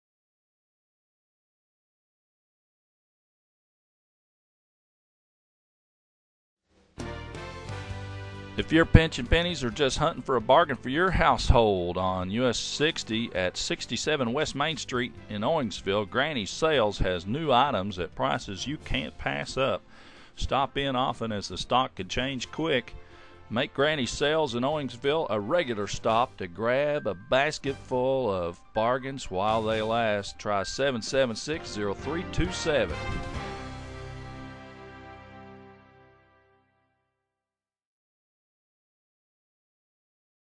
Granny's Sales Owingsville
Great "Outlet" Store on the hill in Owingsville
Better than Grandview they get stuff from
un-named Dept. Store Returns and some New Stuff